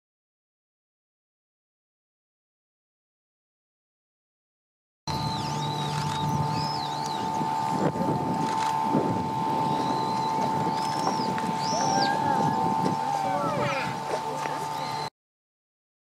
ノビタキ
平塚市の馬入お花畑で。ローズマリーの垣根に止まっています。コスモスを背景に撮りました。
もう冬羽になって見分けにくいのですが、喉が黒くないので雌でしょうか。
FZ200＋TCON-17で撮影。